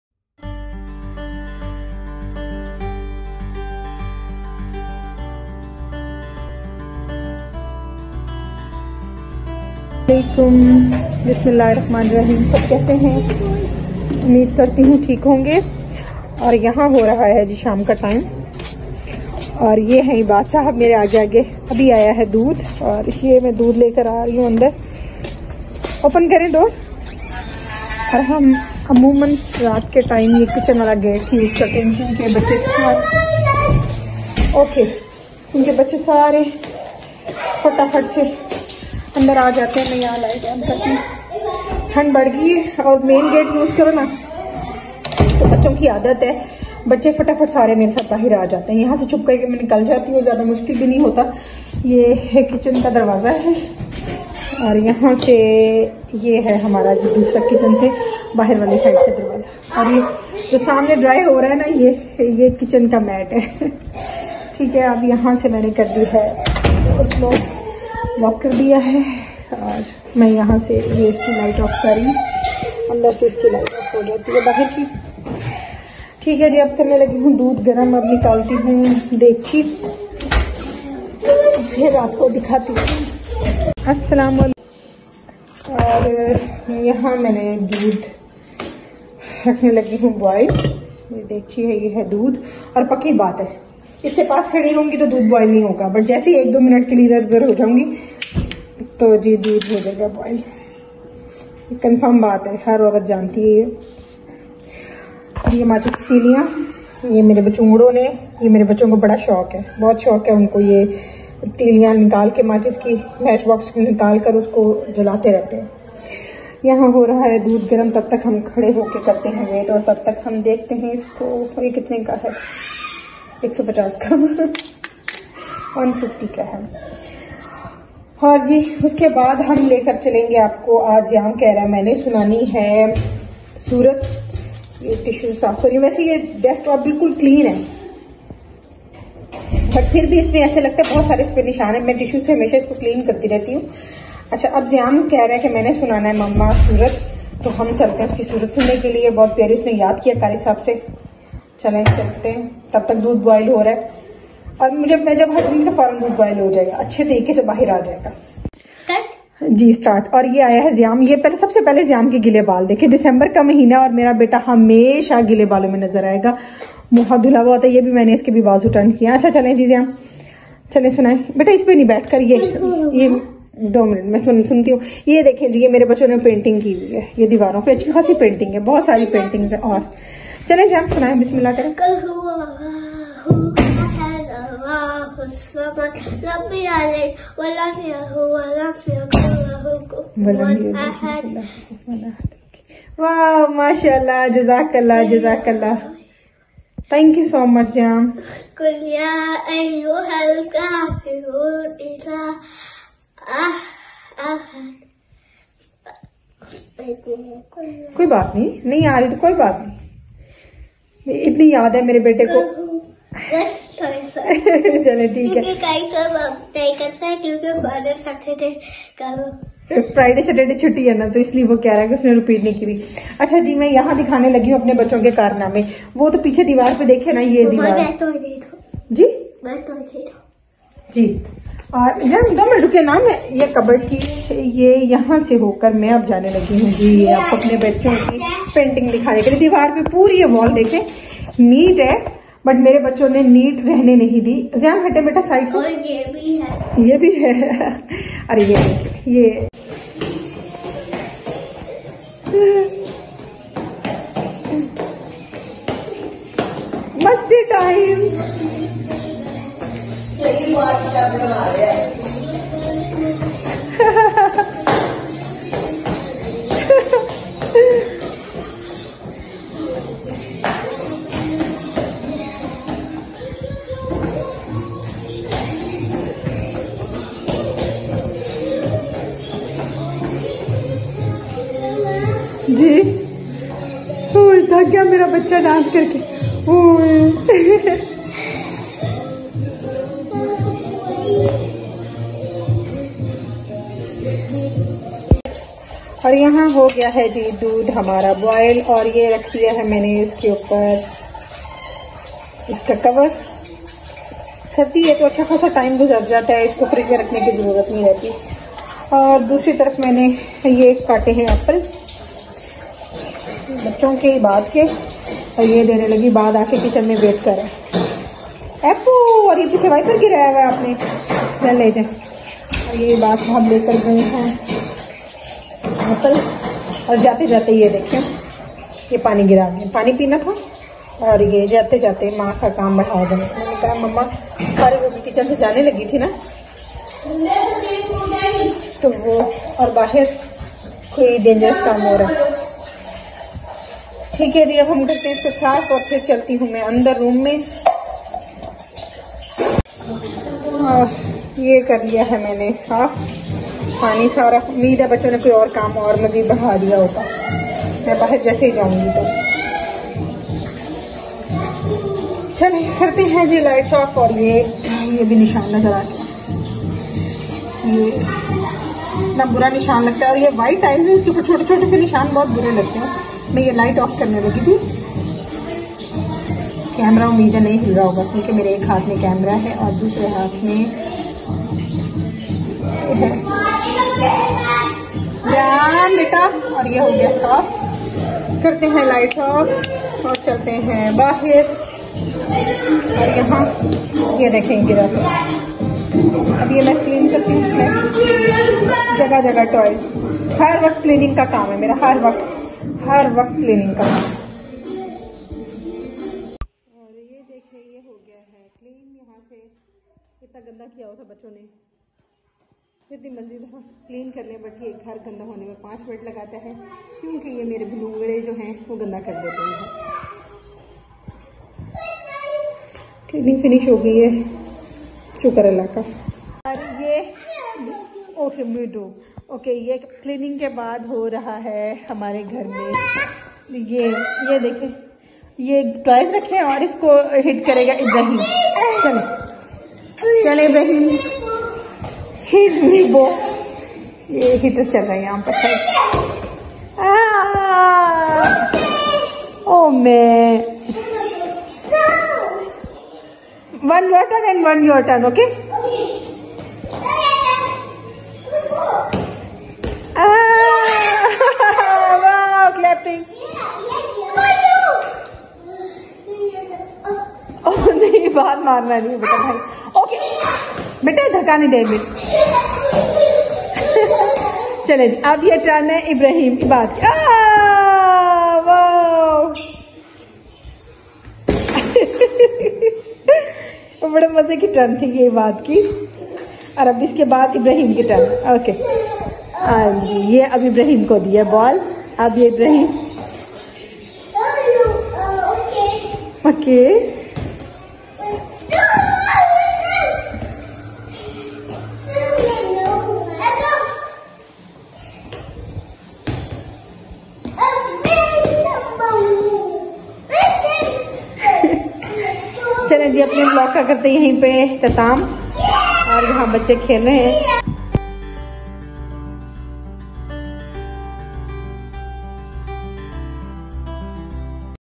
VLOG-04 II EVENING OF WINTERS - WITHOUT HUSBAND - BAHRIA TOWN LAHORE
A SAD EVENING WITHOUT HUSBAND BUT KIDS MADE IT MASTI TIME. SHARING AN EVENING OF WINTERS IN BAHRIA TOWN LAHORE